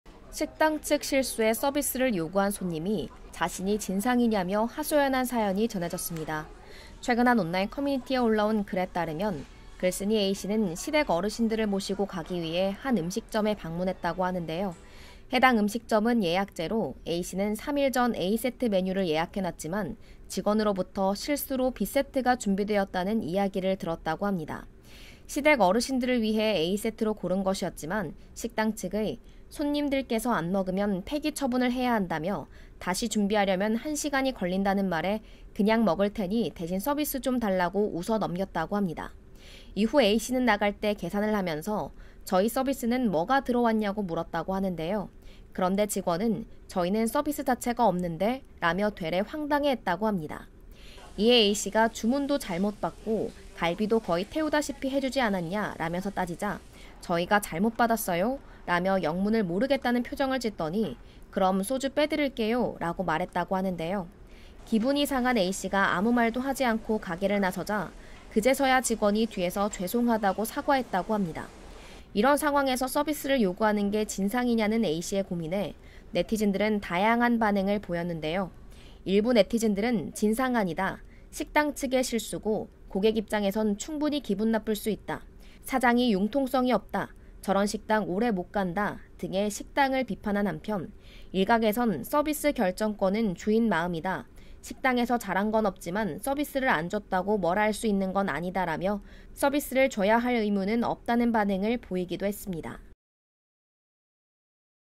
직원 실수로 피해 본 손님 "서비스 달라" 요구했더니... / KNN
최근 한 온라인 커뮤니티에 올라온 글에 따르면 글쓴이 A씨는 시댁 어르신들을
모시고 가기 위해 한 음식점에 방문했다는데요.